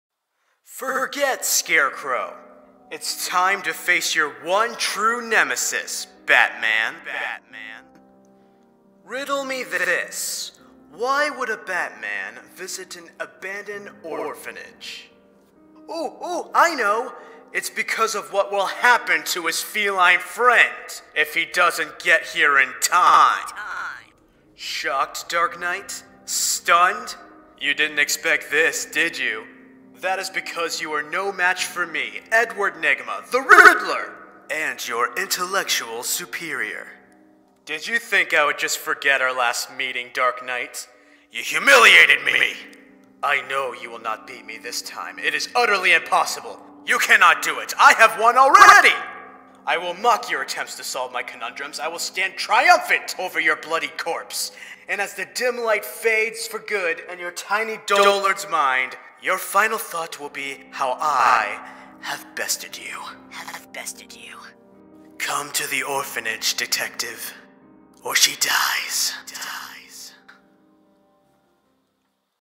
Edward Nigma / The Riddler Voice Impression (Arkham Knight)
After recently getting my own copy of Arkham Knight with my new PS4 last week and was reminded of Wally Wingert's brilliant portrayal of The Riddler, I thought I would give a voice impression of him a shot! I hope you all enjoy ^^